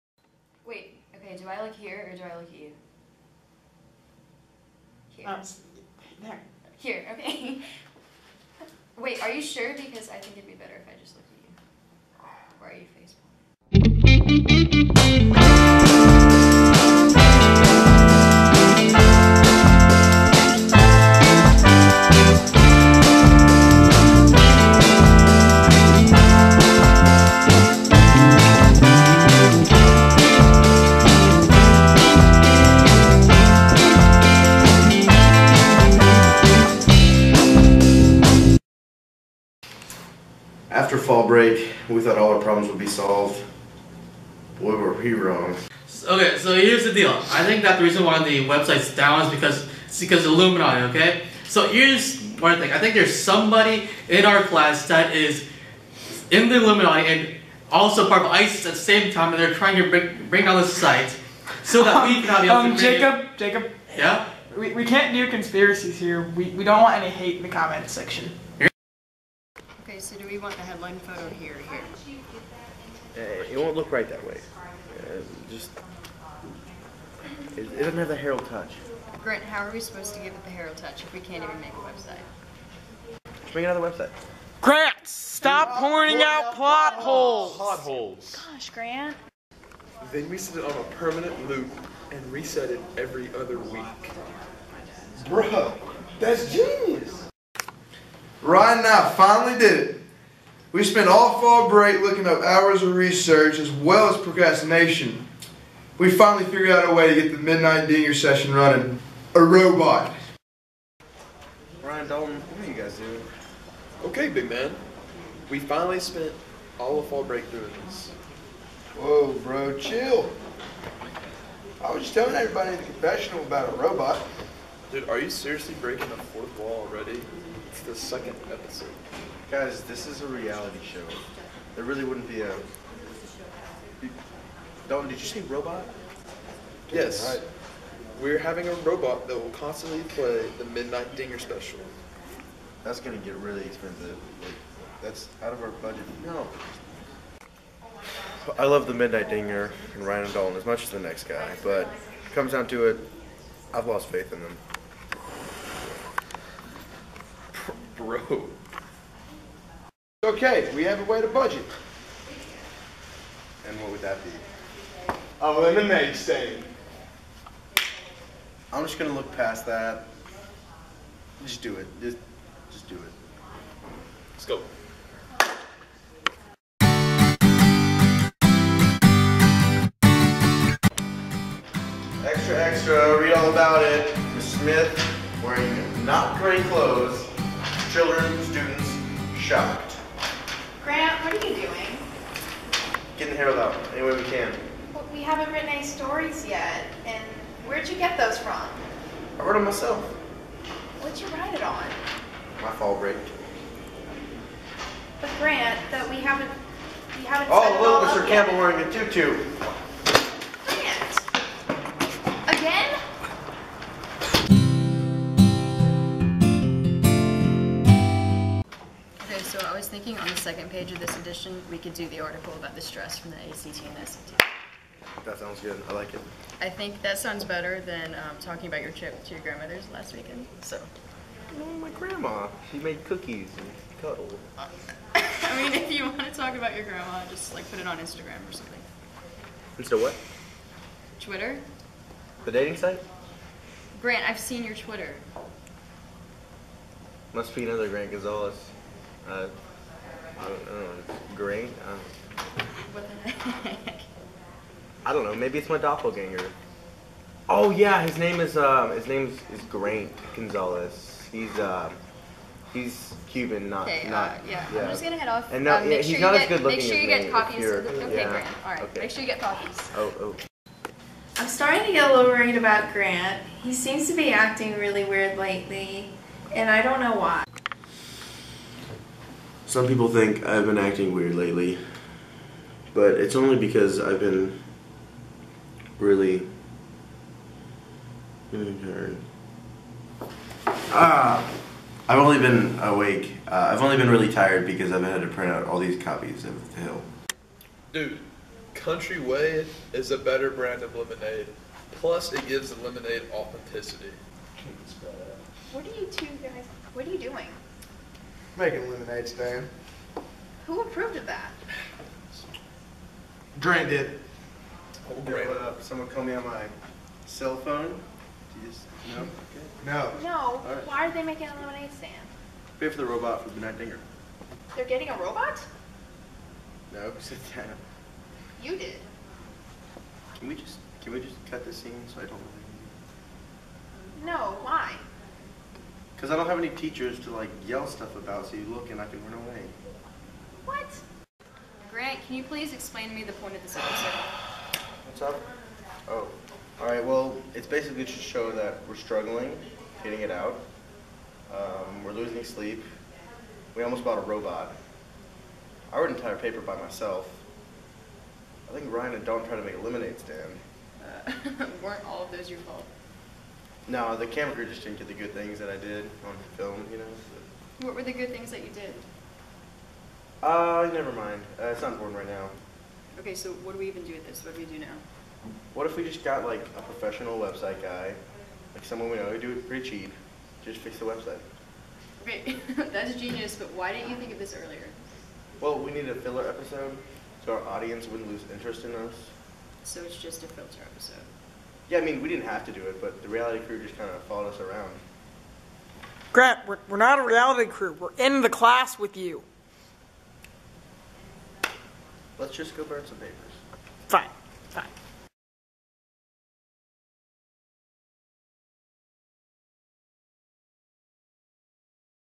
"The Hill" S1E2 "The Struggle"
After returning from Fall Break, the "Herald" crew hoped to return with a fixed website. Boy were they wrong. In this week's edition of "The Hill" the crew has to cope with the struggles that come without a website.  Written and directed by Oliver Antone